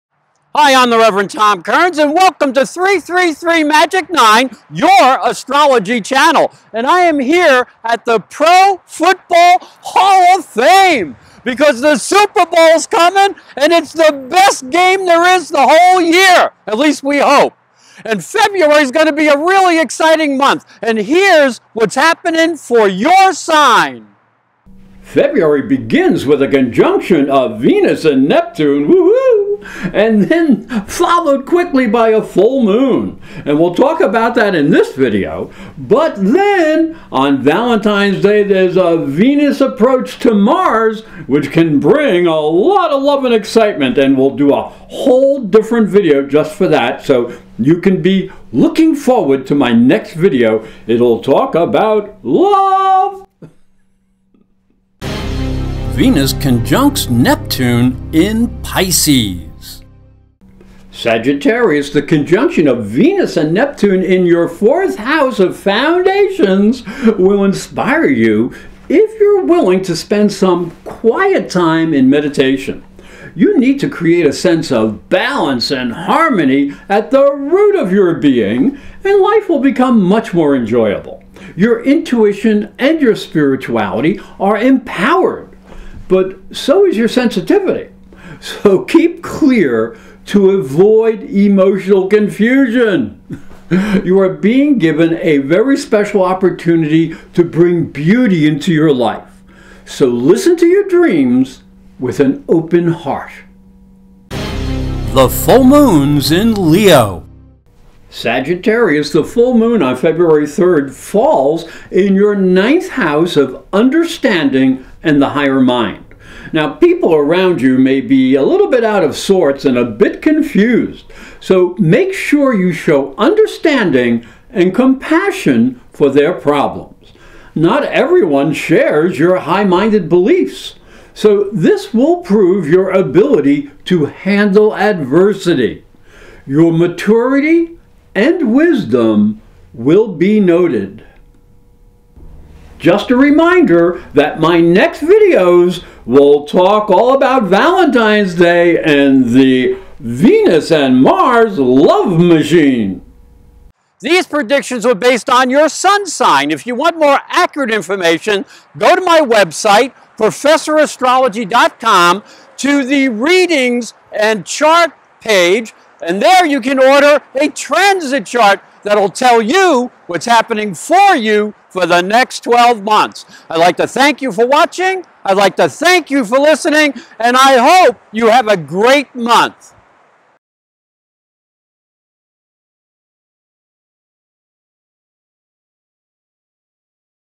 Sagittarius February 2015 Astrology Horoscope
presents Sagittarius Astrology Horoscope February 2015 - Sagittarius, The month begins with a Venus Neptune Conjunction in Pisces and your fourth house of home and family deepening your spiritual roots.  The Full Moon occurs on the third in your ninth house of the higher mind, go to a lecture.  You can register for free astrology predictions at ProfessorAstrology.com This movie was filmed at the Pro Football Hall of Fame in Canton Ohio.